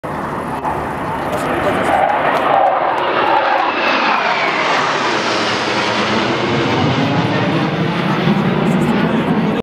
le diamant
Le 21 juillet 2011 la base militaire de Florenne est survolée par cinq F16 revenant du défilé de Bruxelles; J'y était.